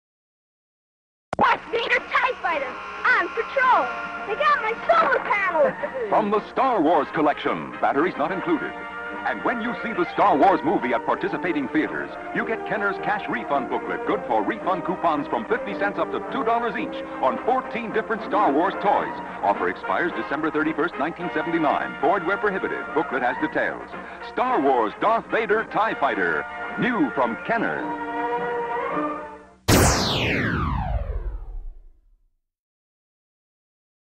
Darth Vader's TIE Fighter & Star Wars Coupon Book Vintage Commercial Kenner
HonorTheForce.com - “When Star Wars happens, we’ll be there!”